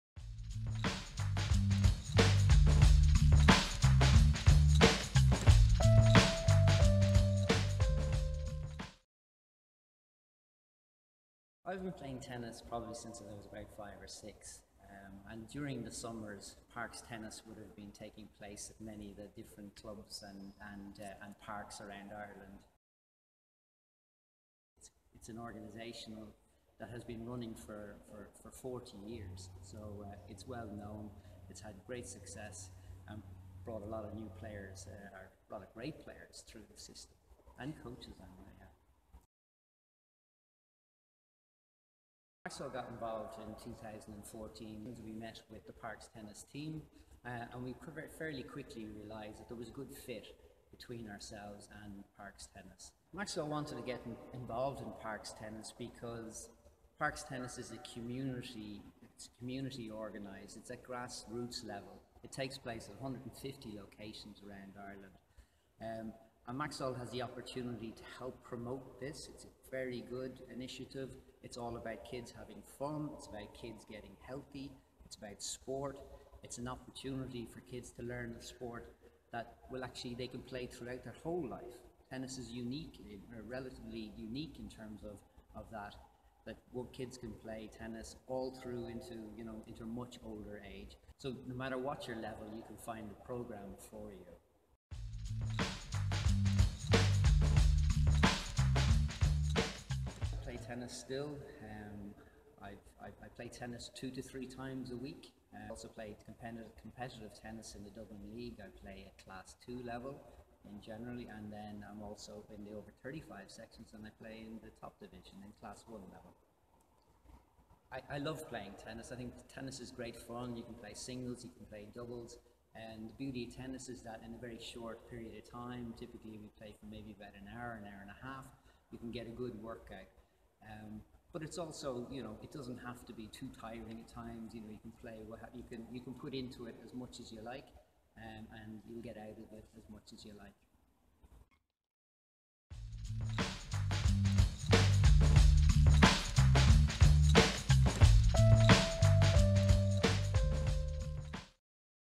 Group Brand Development Manager Fergal Harrington Talks About Maxol Supporting Parks Tennis
Maxol Group Brand Development Manager Fergal Harrington tells us why Maxol is happy to support Parks Tennis Summer Camps. Parks Tennis has 150 venues around Ireland & there's still time to register!